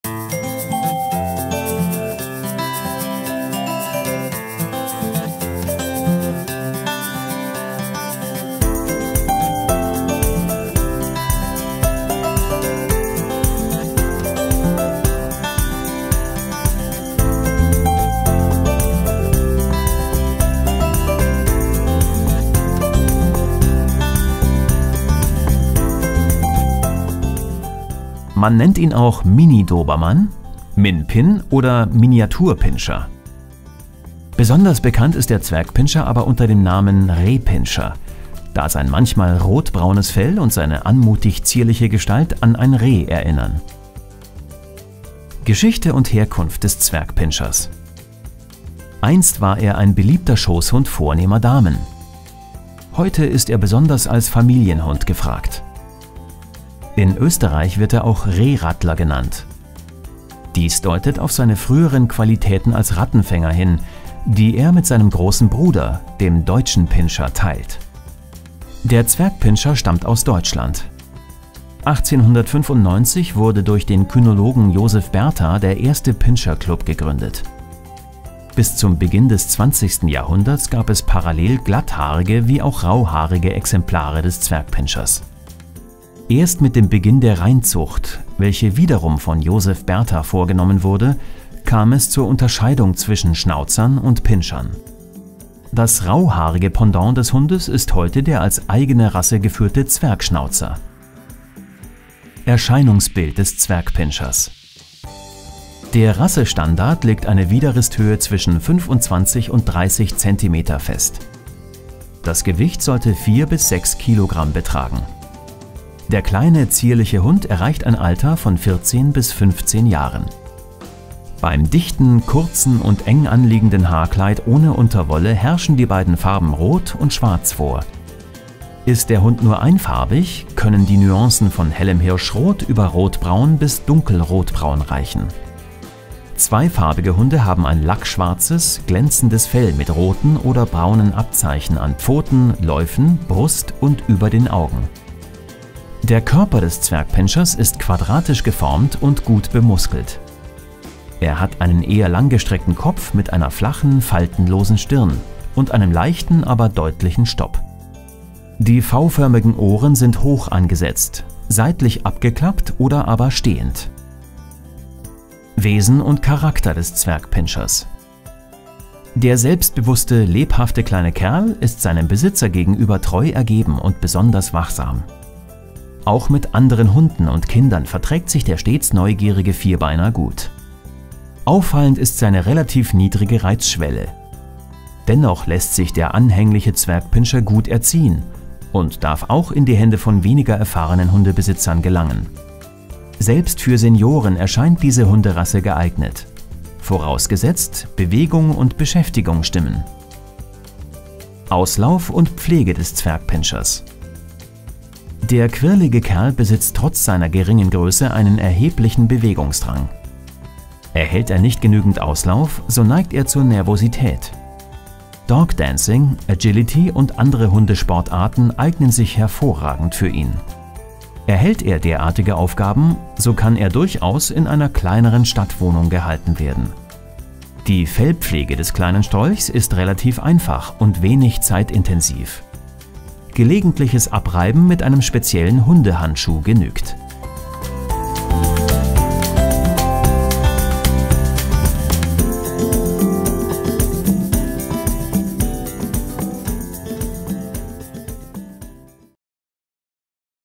► Zwergpinscher [2019] Rasse, Aussehen & Charakter
In dieser Rassebeschreibung findet ihr alle wichtigen Infos zum Zwergpinscher:
► Intro 0:00
► Zwergpinscher Geschichte & Herkunft 0:44
► Zwergpinscher Erscheinungsbild & Aussehen 1:42
► Zwergpinscher Wesen & Charakter 2:44
► Zwergpinscher Auslauf, Pflege & Krankheiten 3:20

Zwergpinscher Rasseportrait [2019] Informativer Praxisratgeber
Cooles Zwergpinscher T-Shirt
Süße Zwergpinscher Tasse